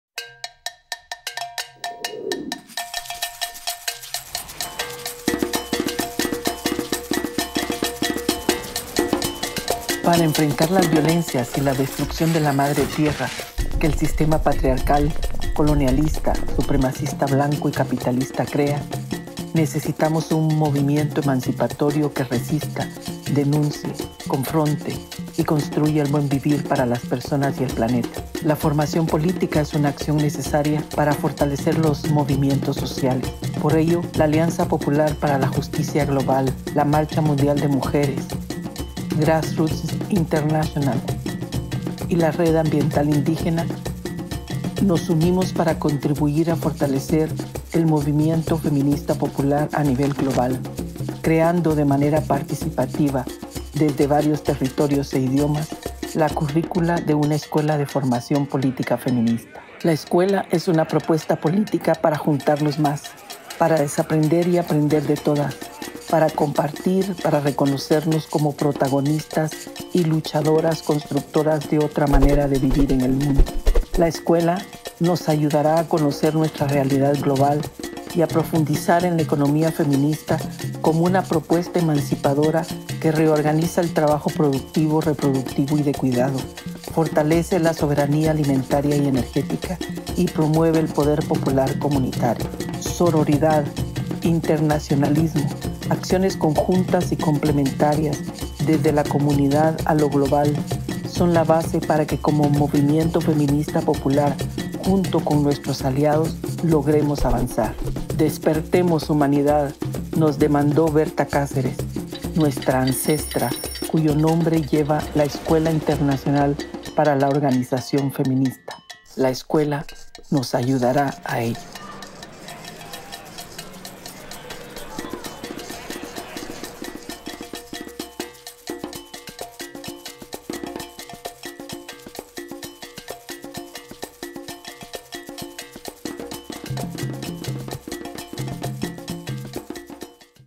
[ES/EN/FR/PT] Escuela Internacional de Organización Feminista "Berta Cáceres"
Evento virtual rindió homenaje a las luchas de las mujeres en los territorios y marcó la importancia de la formación para la organización internacional del feminismo popular // Online event paid tribute to women’s struggles in their territories and illustrated the importance of education for grassroots feminist international organizing // Événement en ligne a rendu hommage aux luttes des femmes dans les territoires et a marqué l'importance de la formation pour l'organisation internationale du féminisme populaire.